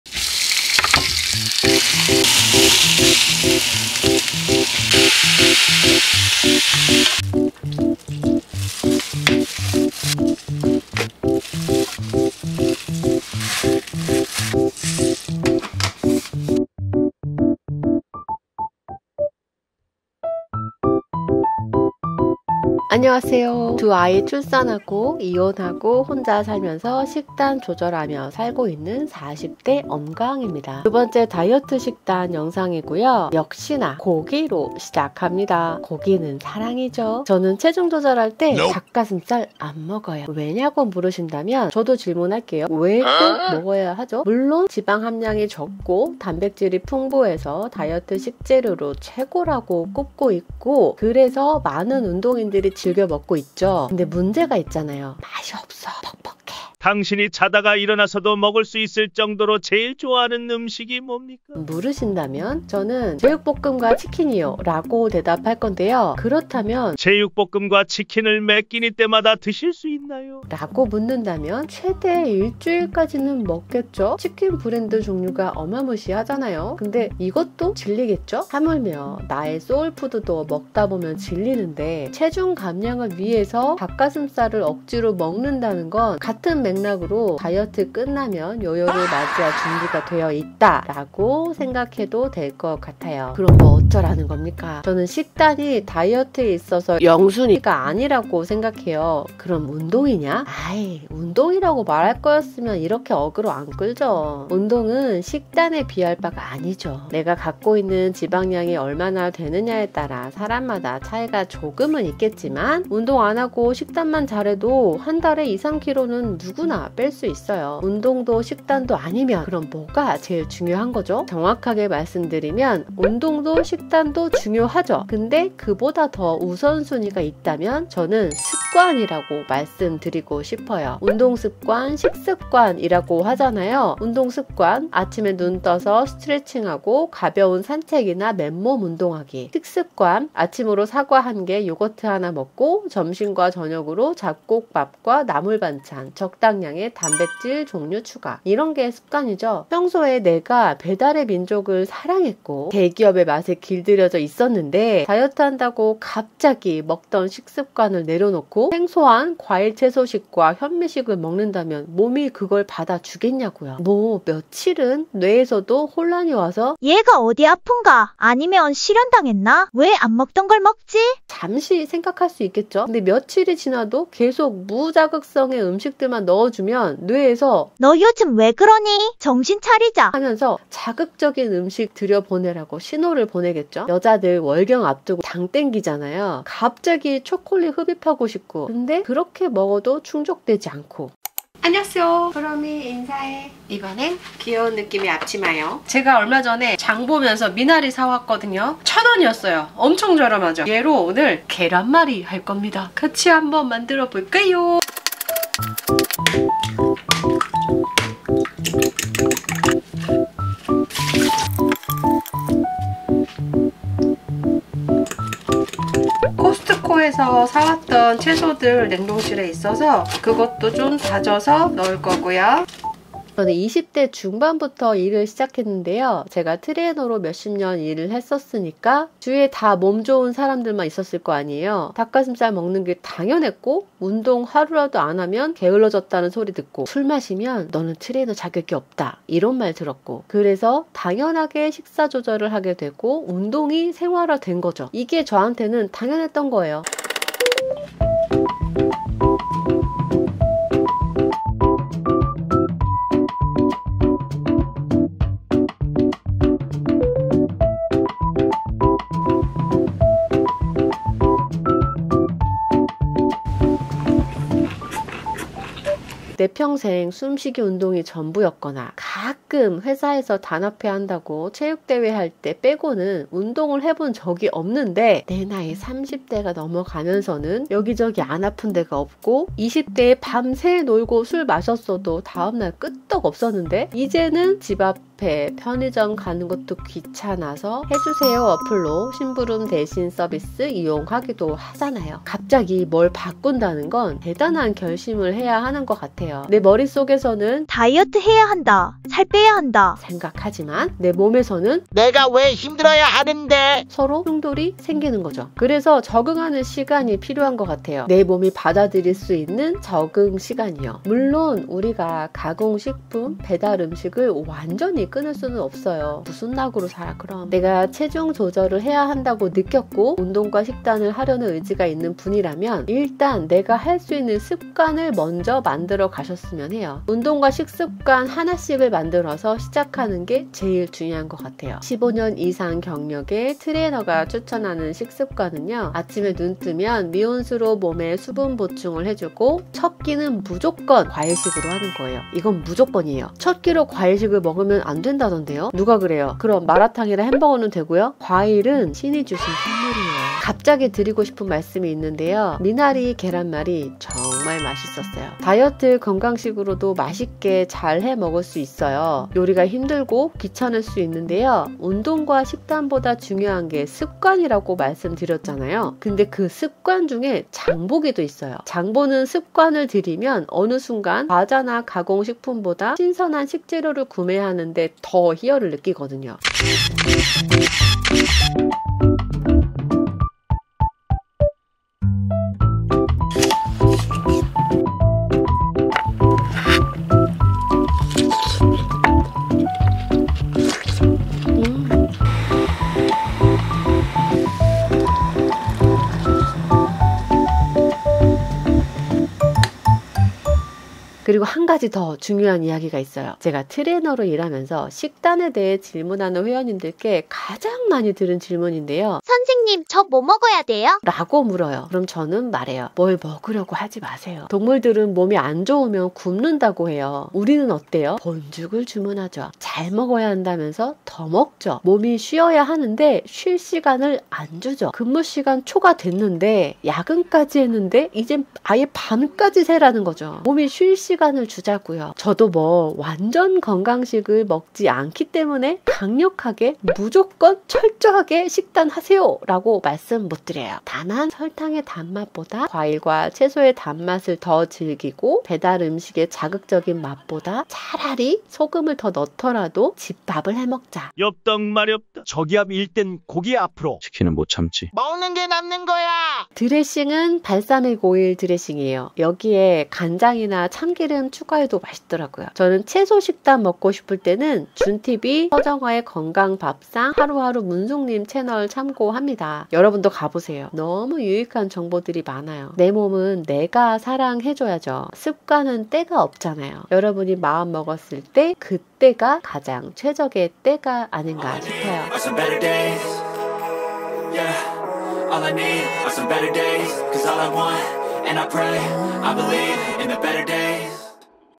[이혼-일상] 운동과 식단보다 중요한 것!! 중년 다이어트 식단은 달라야 합니다
♥영상 제작 후원♥

안녕하세요. 엄강이에요.
오늘은 중년의 나이에도 건강을 위해서
다이어트를 해야 하는데
그럼 어떻게 관리 해야 하는지에 대해서
이야기 해볼게요.
식단 전문가는 아니지만
구독자님들께 도움 될 만한 내용들
담아 봤어요.

처음부터 보시길 권장 드리고
이야기가 연결되기 때문에
타임라인은 안 했답니다~~~
양해 부탁드려요~~~😅😅😅

재미있게 시청해주시면 감사하겠습니다!!!